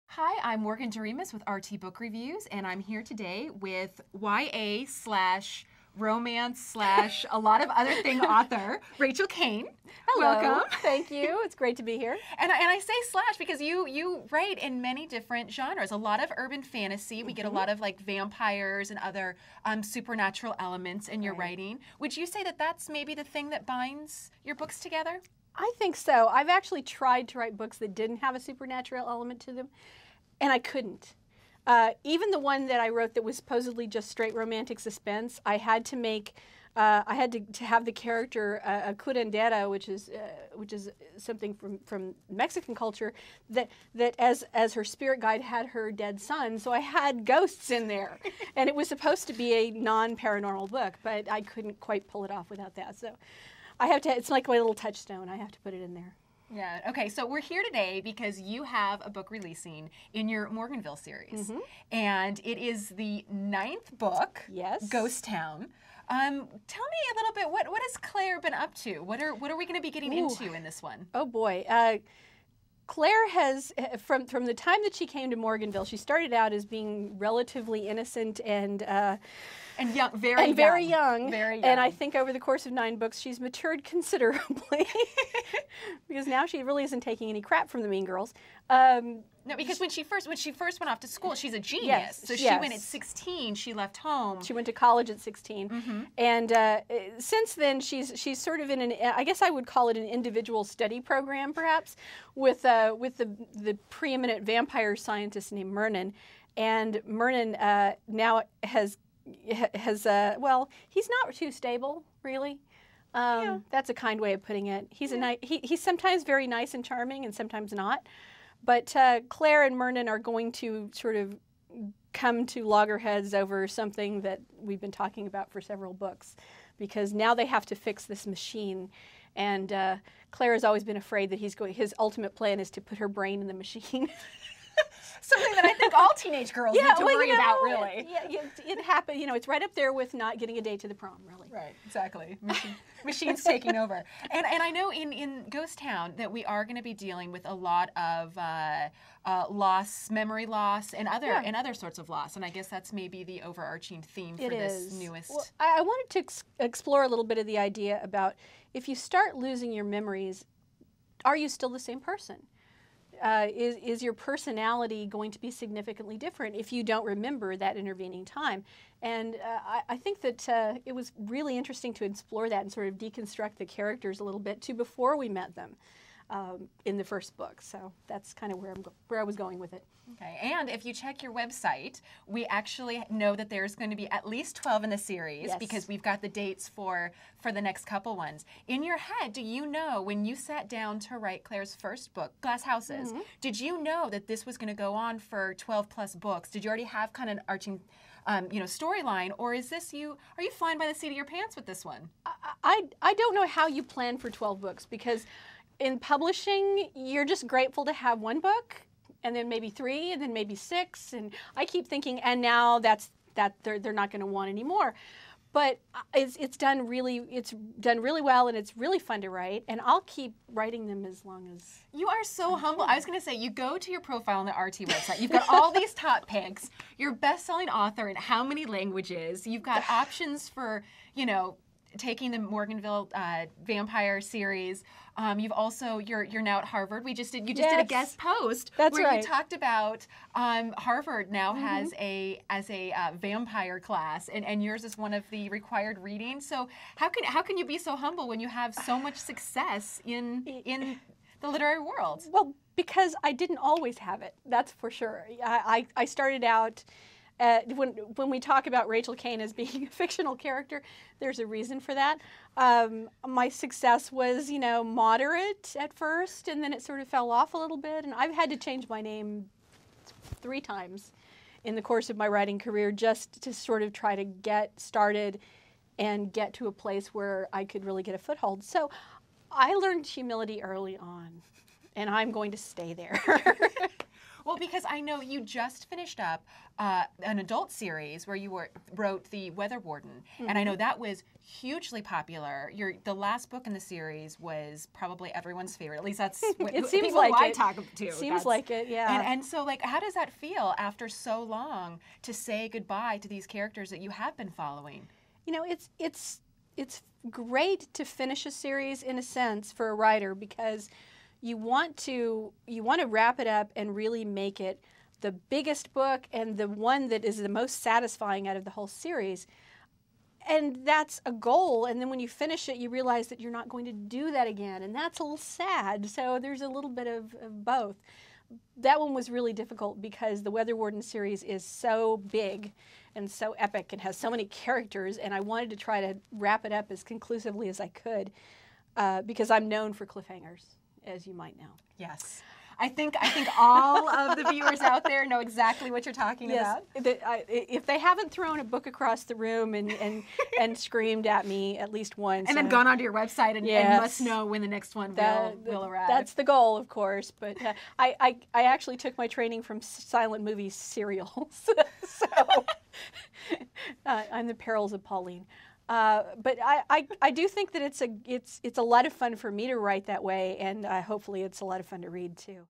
RT Interviews Author Rachel Caine
RT BOOK REVIEWS Magazine interviews author Rachel Caine about her writing career, the ending of the Weather Warden series and her latest release, Morganville Vampire tale Ghost Town.